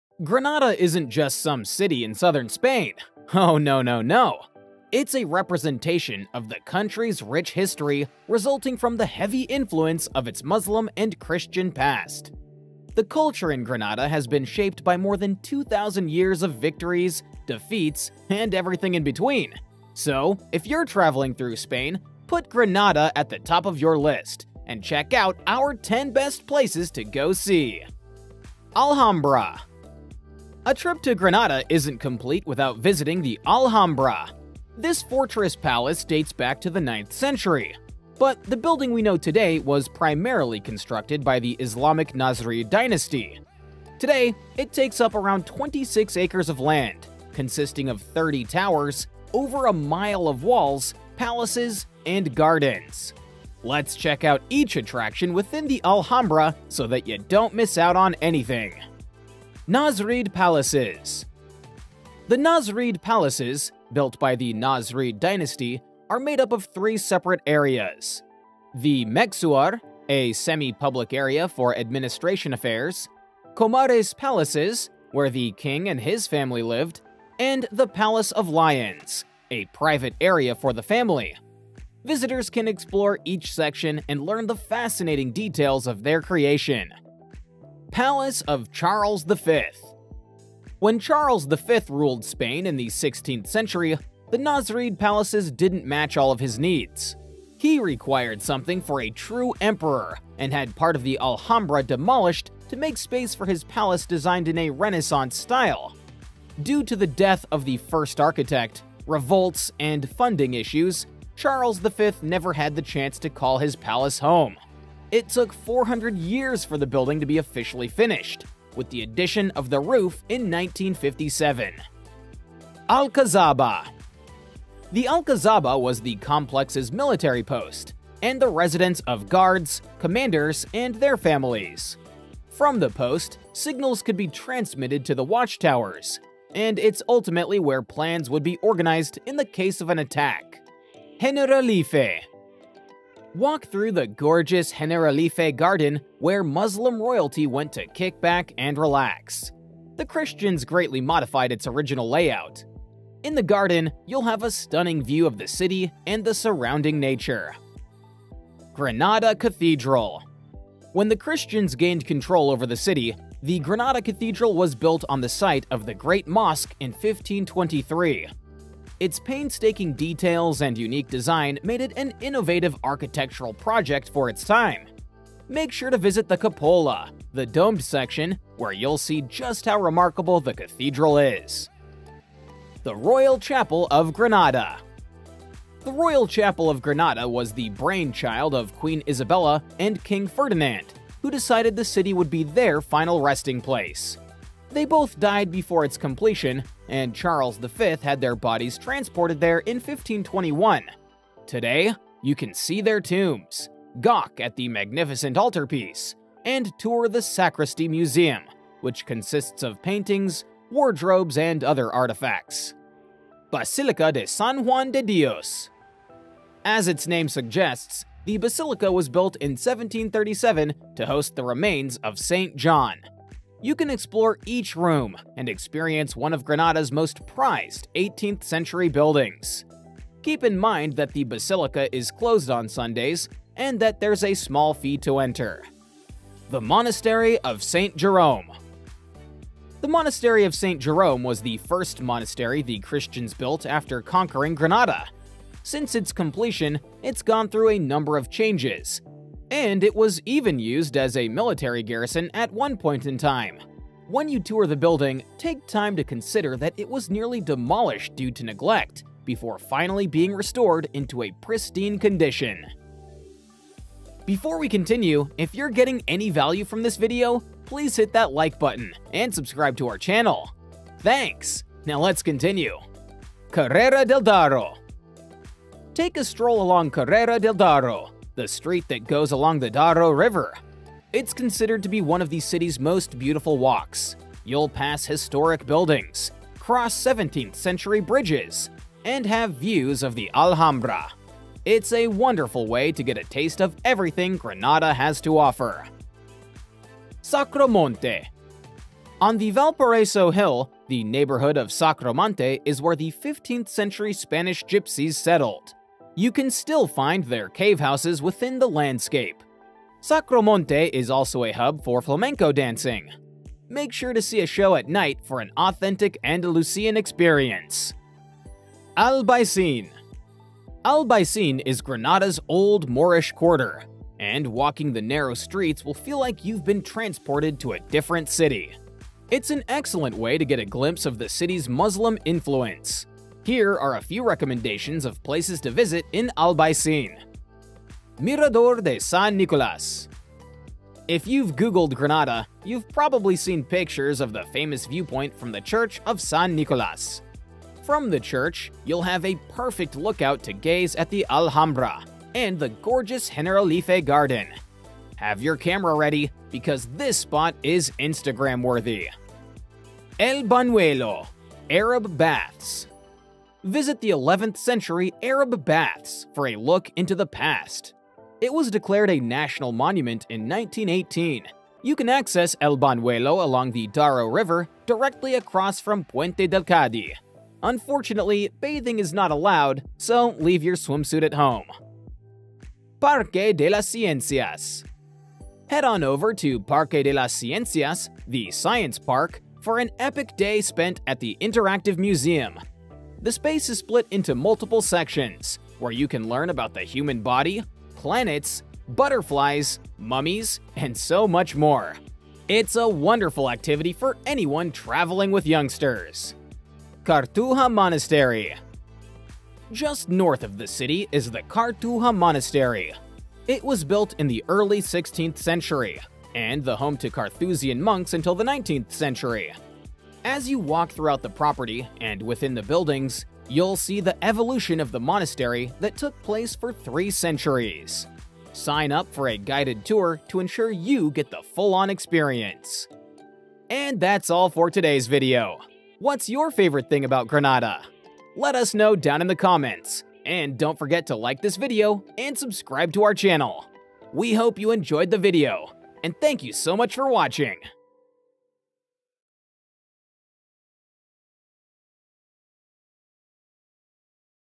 10 BEST PLACES TO SEE IN GRANADA - Spain Travel Guide
In this video, we'll share our top 10 places to see in Granada, the wonderful city in southern Spain. From the stunning Alhambra Palace to the serene Andalusian landscapes, this is a list of must-see attractions in Granada.

If you're planning a trip to Spain, be sure to check out this 10 best places to see in Granada! This guide will help you plan your trip and choose the attractions that are the best fit for your interests and travel style. From history to architecture to food, this city has it all!

Check out some more videos from our channel:-